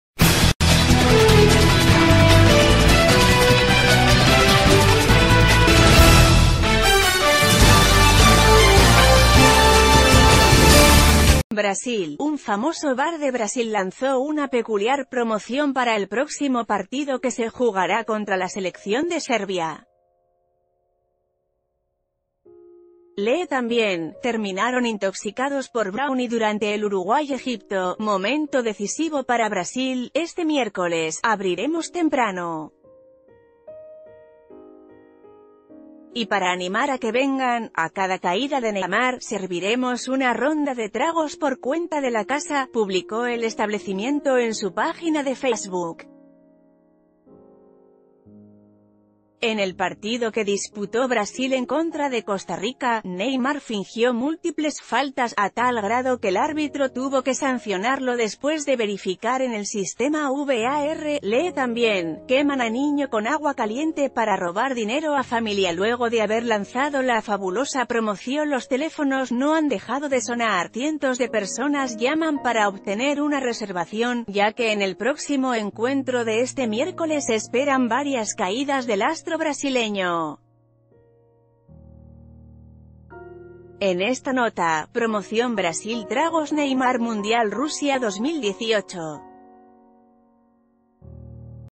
Bar regalará tragos cada vez que Neymar se tire al suelo
Brasil.- Un famoso bar de Brasil lanzó una peculiar promoción para el próximo partido que se jugará contra la selección de Serbia. Lee también: Terminaron intoxicados por brownie durante el Uruguay-Egipto“¡Momento decisivo para Brasil!, este miércoles, abriremos temprano. Y para animar a que vengan,...